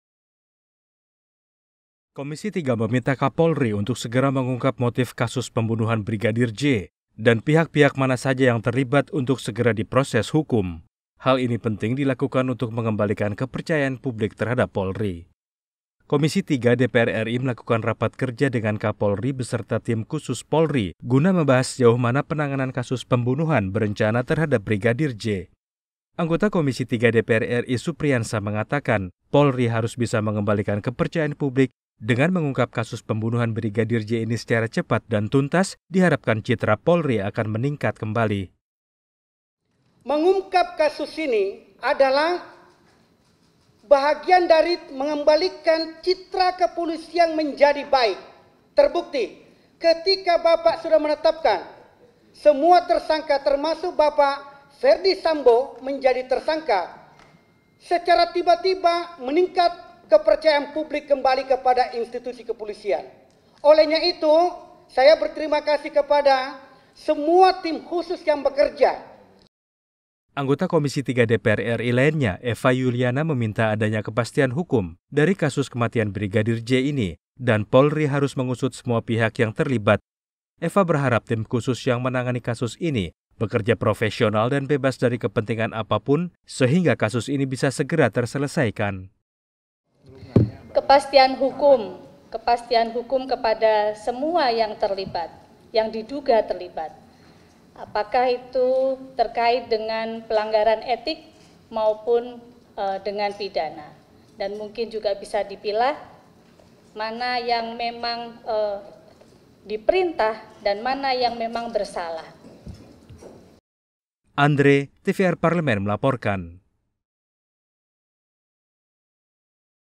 TVR 17 - Timsus Polri Diharapkan Bekerja Profesional dan Transparan
Komisi III meminta Kapolri untuk segera mengungkap motif kasus pembunuhan brigadir J dan pihak - pihak mana saja yang terlibat untuk segera diproses hukum. Hal ini penting dilakukan untuk mengembalikan kepercayaan publik terhadap Polri.

Narasumber :

Supriansa / Anggota Komisi III DPR RI / F – PG / Sulsel II

Eva Yuliana / Anggota Komisi III DPR RI / F – PNasdem / Jateng V